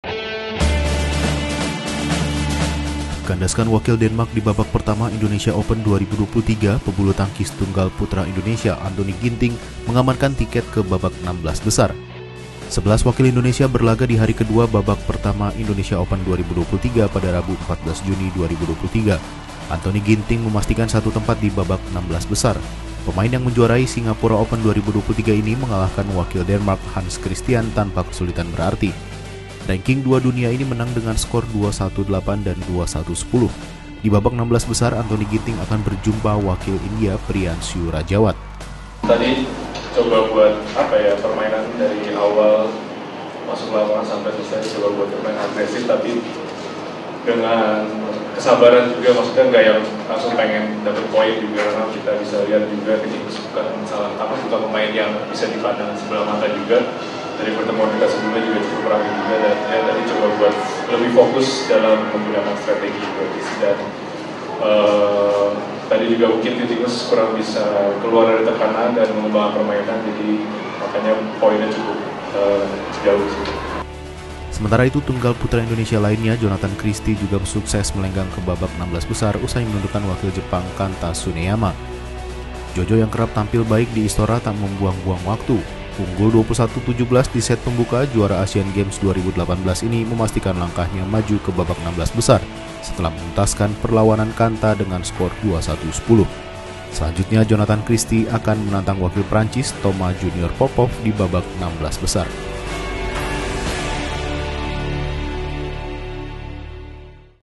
Tunggal Putra Indonesia Raih Tiket ke Babak 16 Besar Indonesia Open 2023
Kandaskan wakil Denmark di babak pertama Indonesia Open 2023, pebulu tangkis tunggal putra Indonesia Anthony Ginting mengamankan tiket ke babak 16 besar.